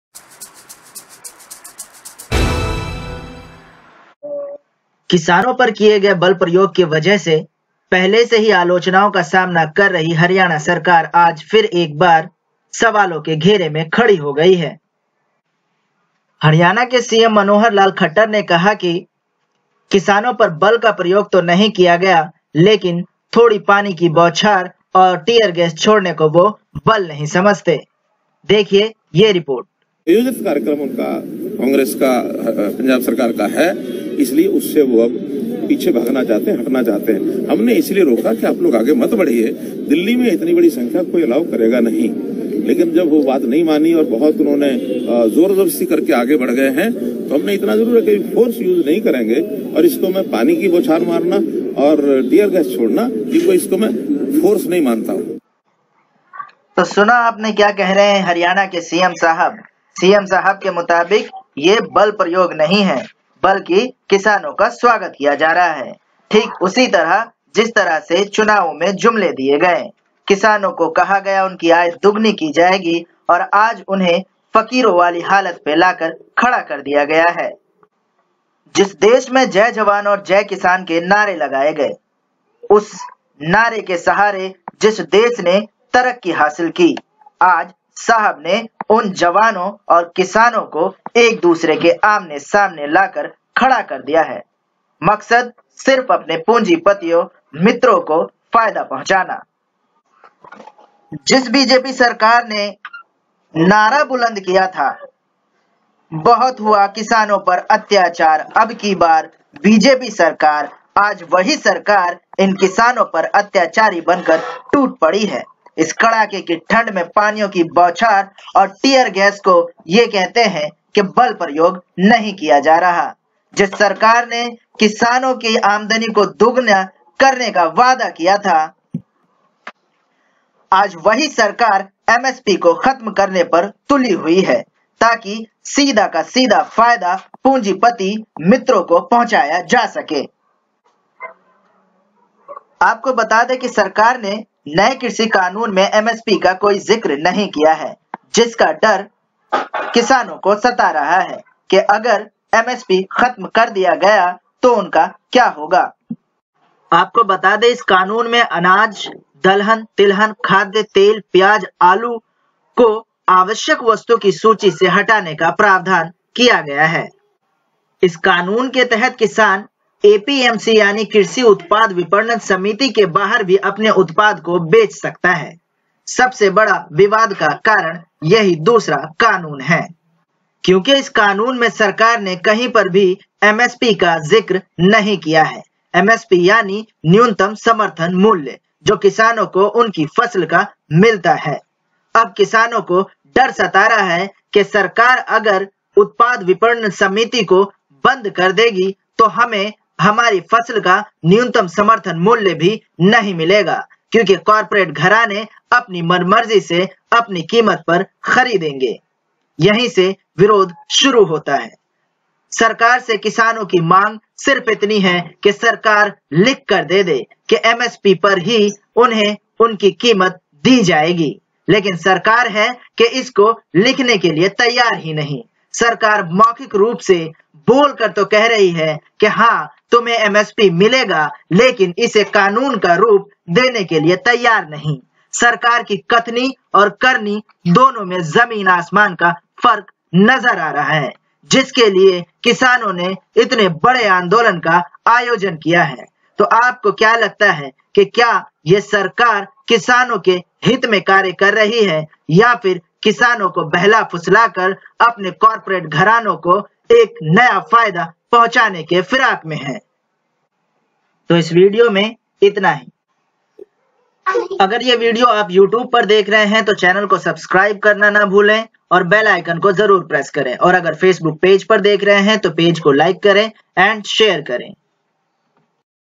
हरियाणा के सीएम ने कहा किसानों पर आनंसू गैस छोड़ना बल उपयोग नहीं है
किसान मोदी सरकार के खिलाफ क्यों हैं। नया किसान बिल क्या है और किसान इस बिल विरोध कर रहे हैं मोदी सरकार ! जानिए इस वीडियो में पूरी जानकारी।